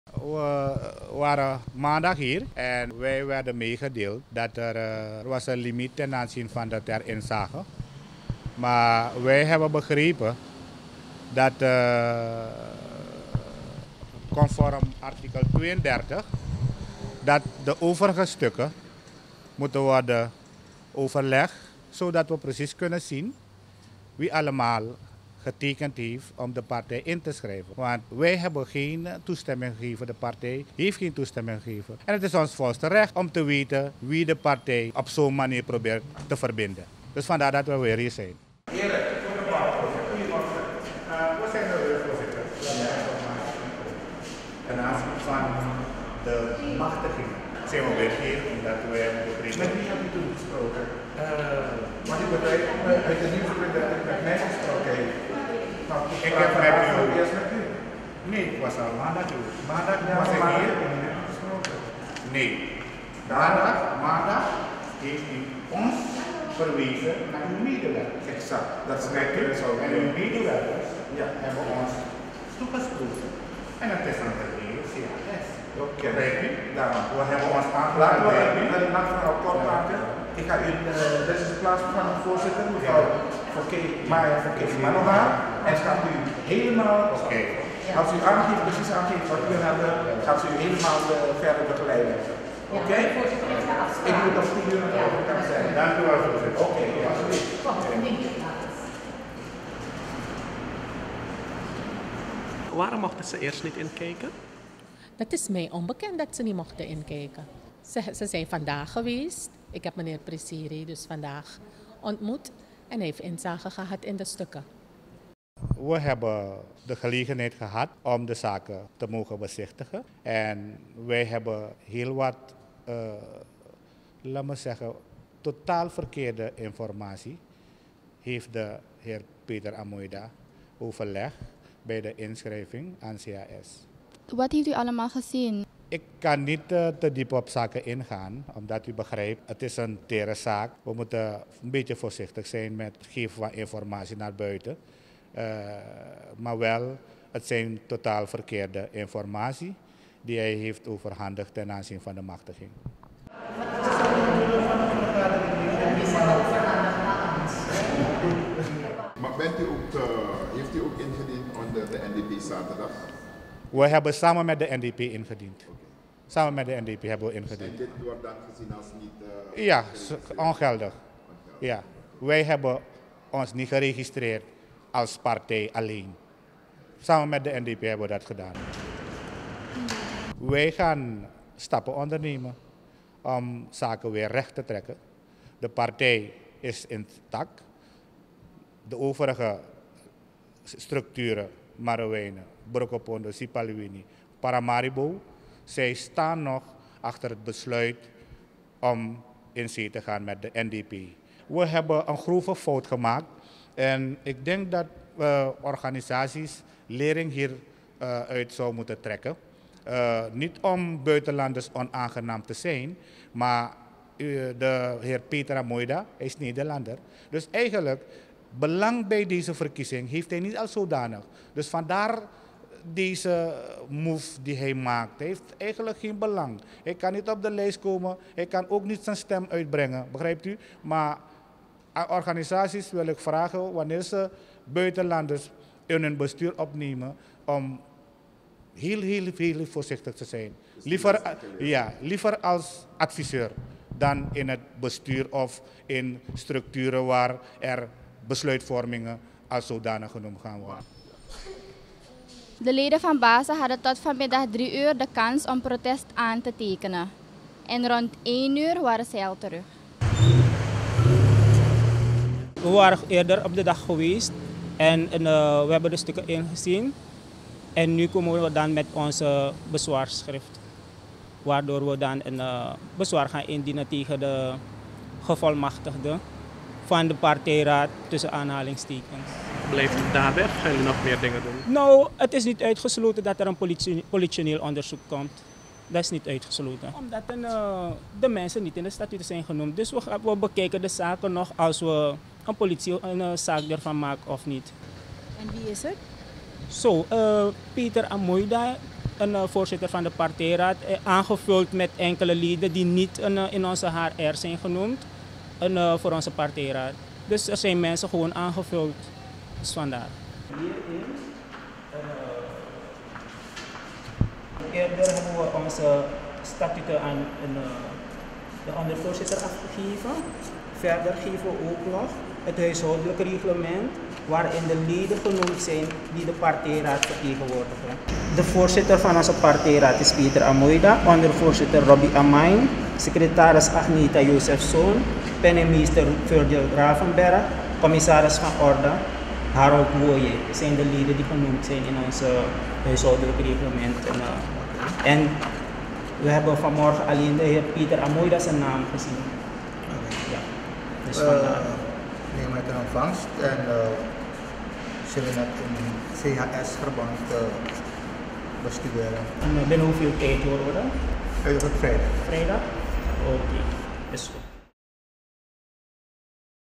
Baasa-voorzitter: Verkeerde informatie bij CHS
Het bestuur van Baasa heeft vandaag tóch toestemming gekregen van het Centraal Hoofdstembureau (CHS) om de stukken waarmee de partij geregistreerd is, in te kijken. Maandag werd aan de bestuursleden, onder leiding van Nicolaas Priesierie, meegedeeld dat het niet mogelijk was. Zij mochten alleen de naam en het symbool zien. Priesierie zegt aan Starnieuws dat op basis van verkeerde gegevens partijraadsvoorzitter Peter Amoida Baasa heeft ingeschreven voor deelname aan de verkiezingen. Er is formeel protest hiertegen aangetekend.